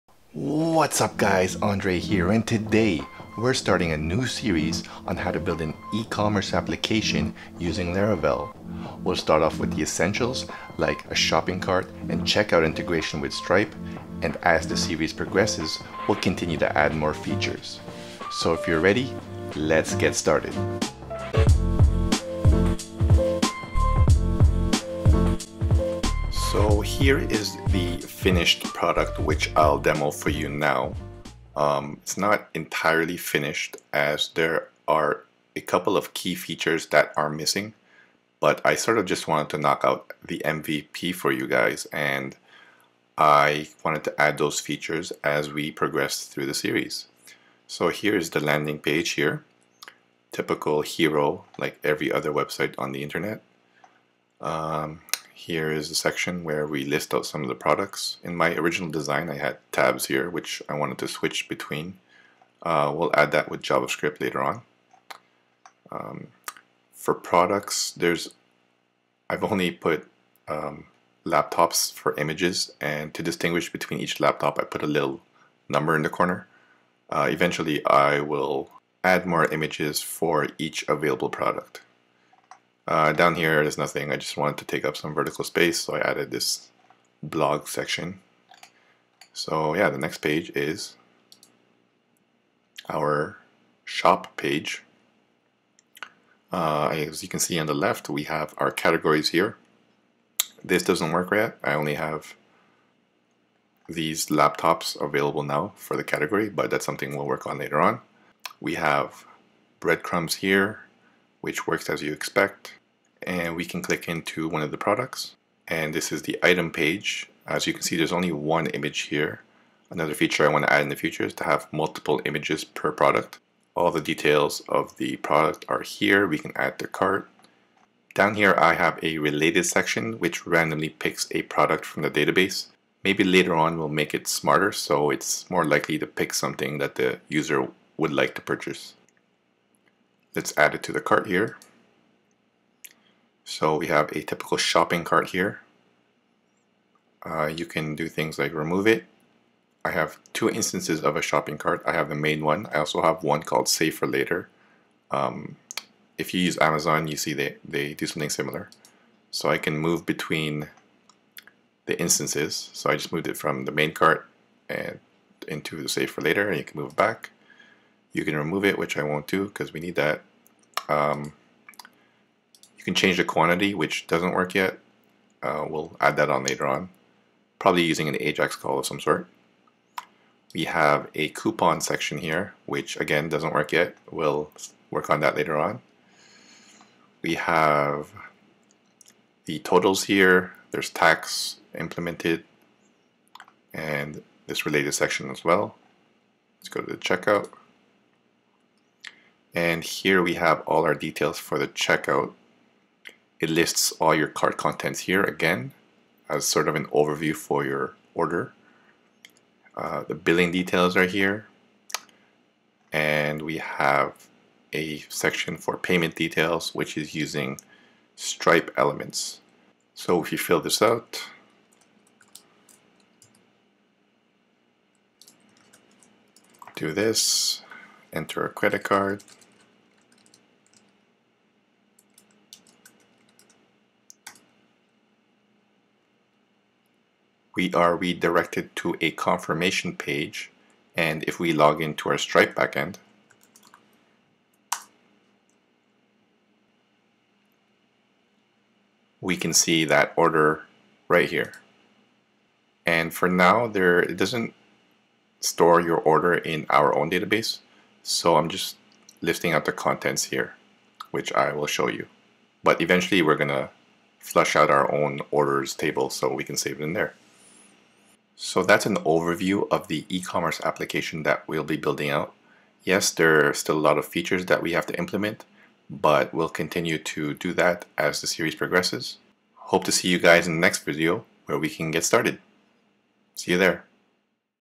Laravel E-Commerce - Introduction
In this series, we'll build a Laravel E-Commerce application with a shopping cart and checkout functionality with Stripe. We'll continue to add more features as the series progresses!